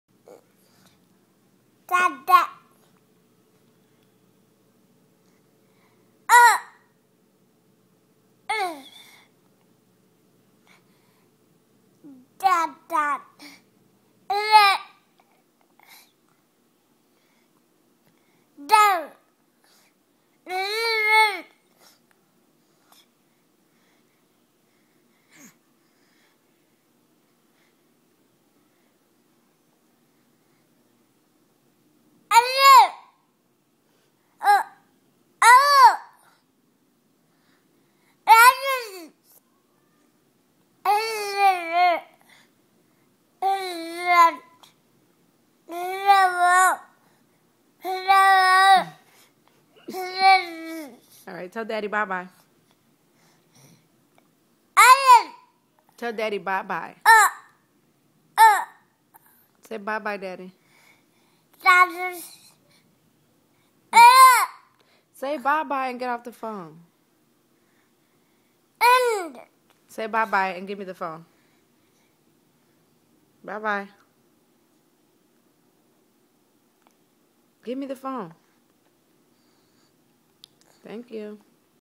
Reidan talks on the phone to daddy
Baby thinks he's saying real words while talking on the phone.